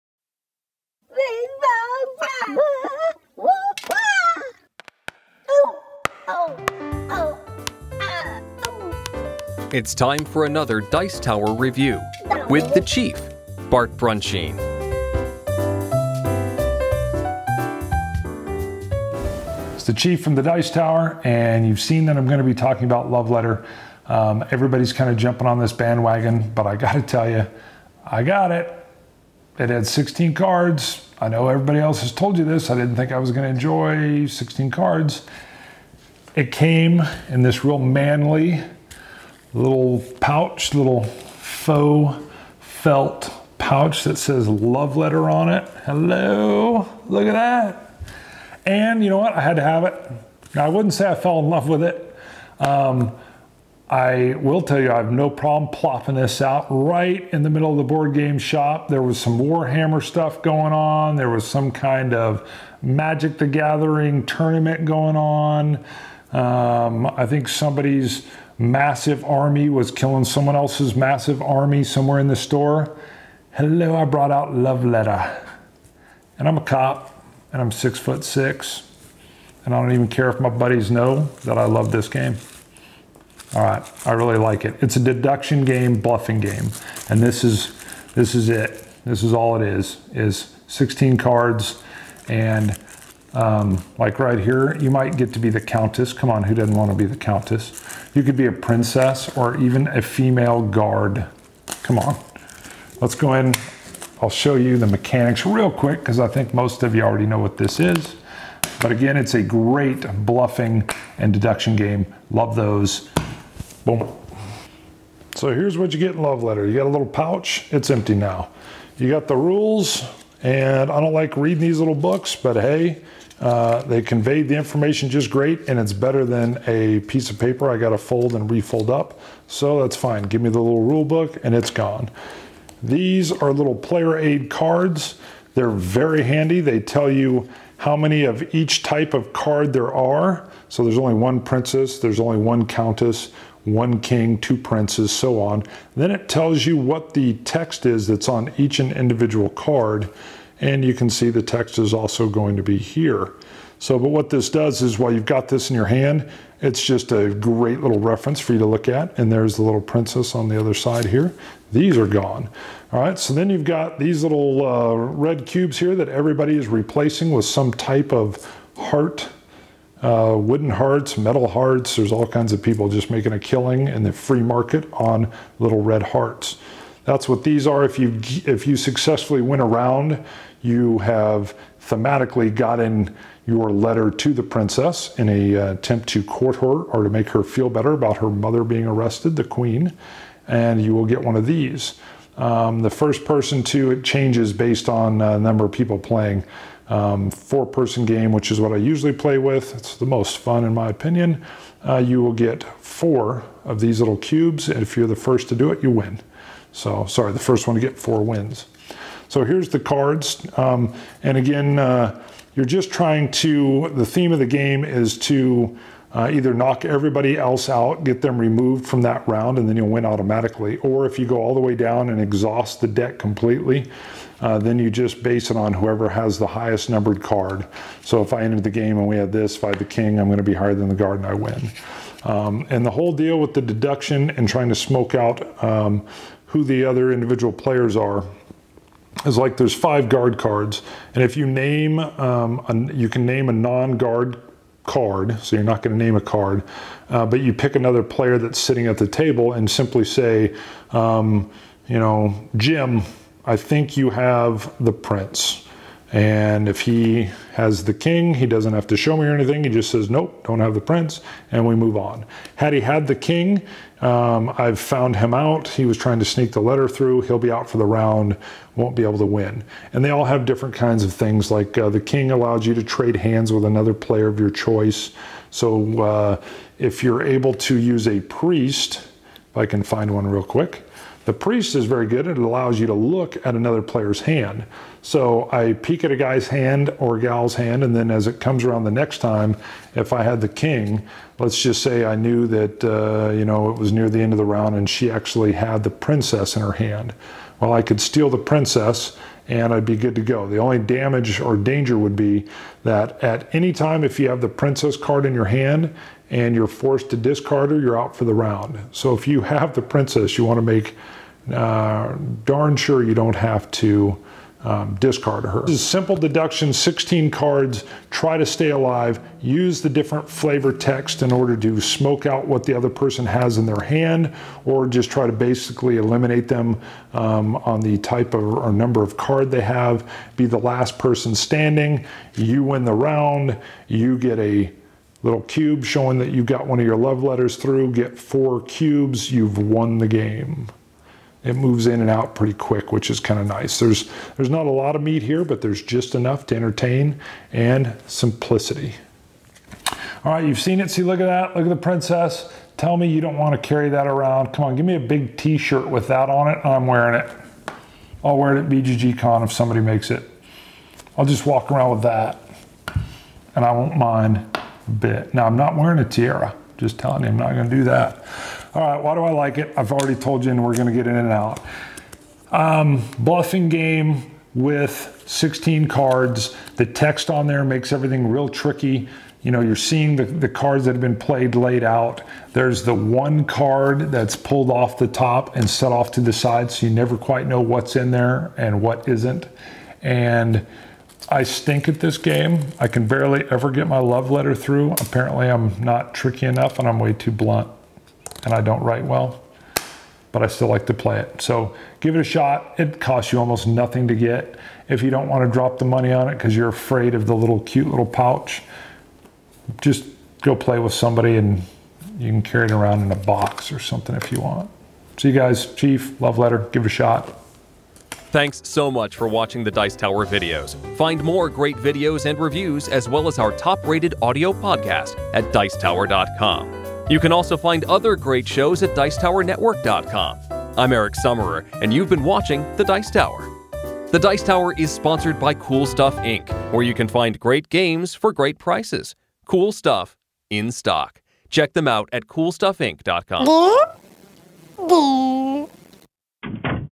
Love Letter Review - with the Chief
Bart examines this small card party game